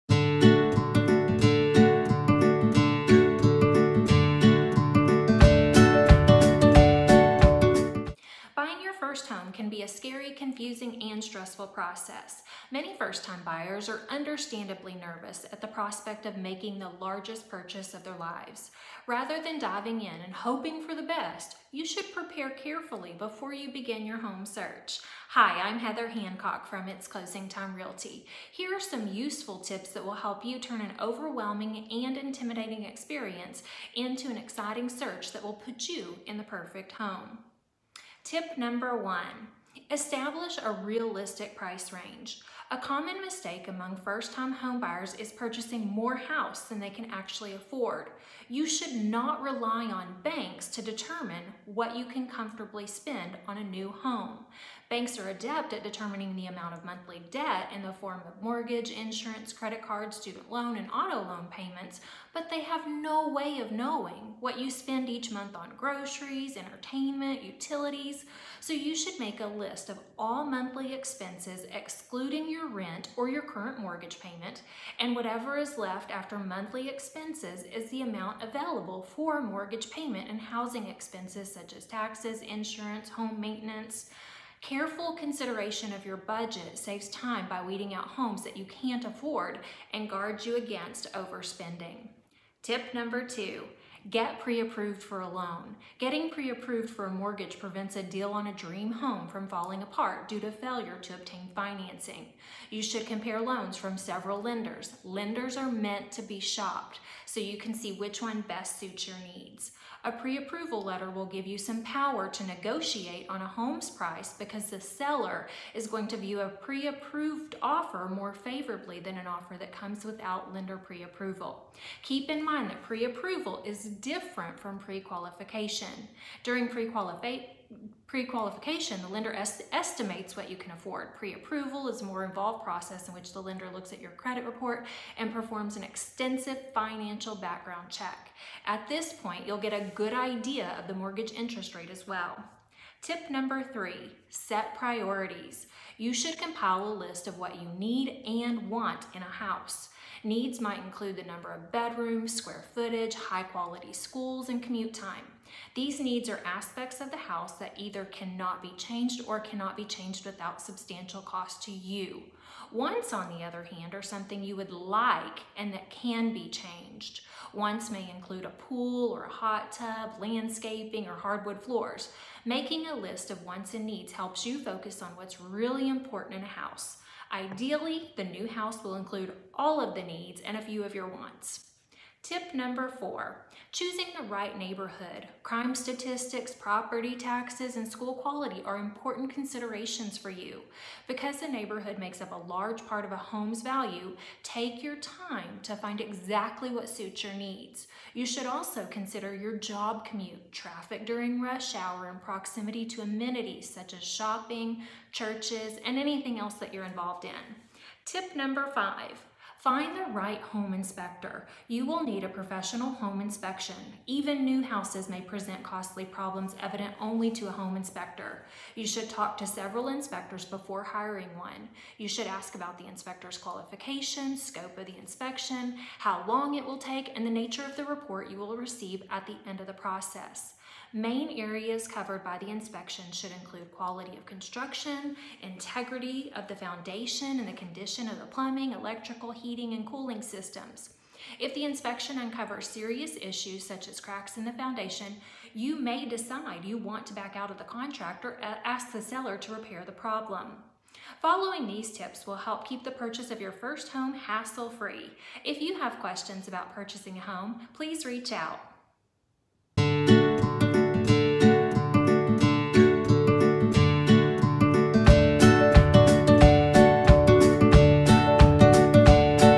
Five Tips for First Time Home Buyers
Buying your first home can be a scary, confusing and stressful process. Many first time buyers are understandably nervous at the prospect of making the largest purchase of their lives. Rather than diving in and hoping for the best, you should prepare carefully before you begin the house search. Use these tips to make your home buying process hassle free!

I'm a full time REALTOR® in the Dallas/Fort Worth area.

TrophyRealtyGroup.com